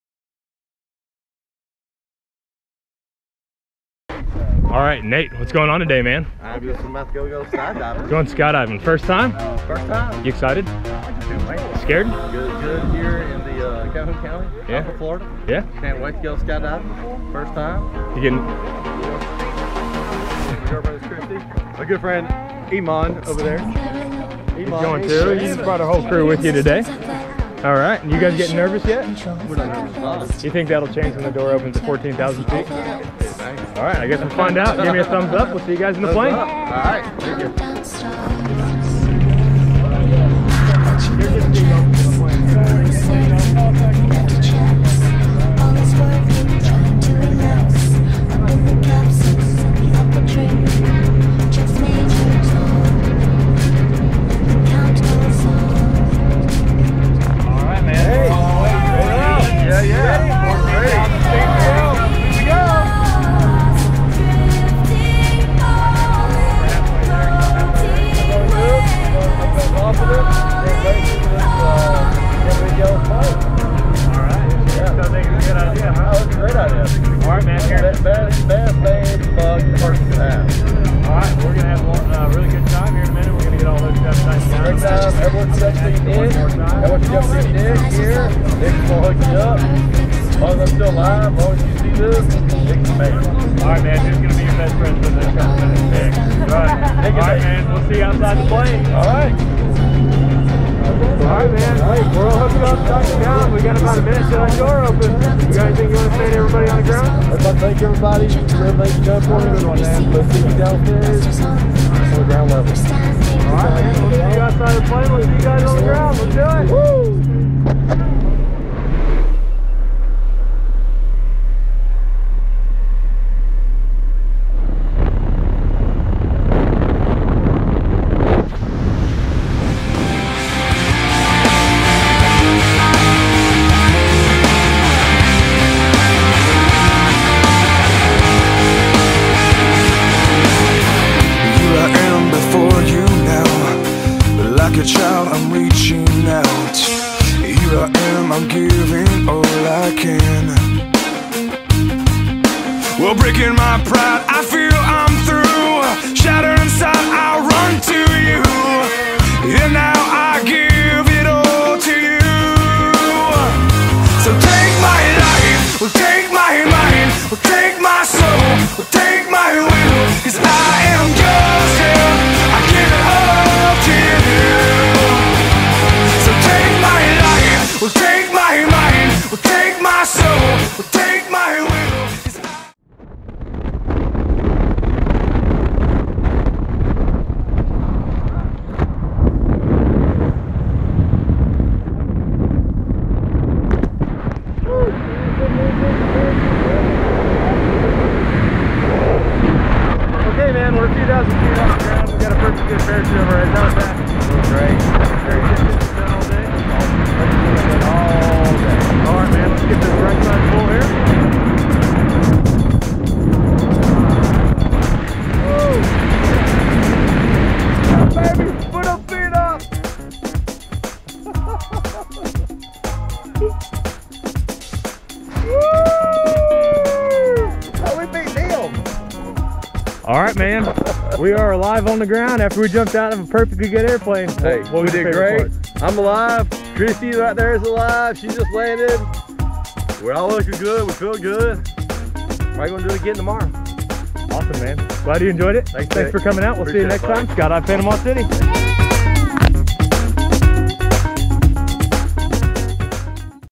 Nathan - Jumps at Skydive Panama City
Nathan - makes a jump at Skydive Panama City